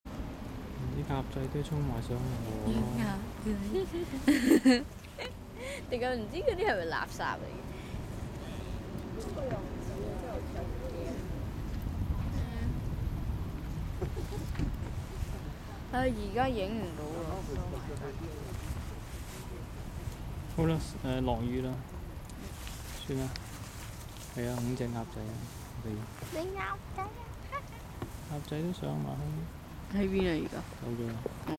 20090824 7 5Ducks
day 6 five ducks get pushed along by the high tide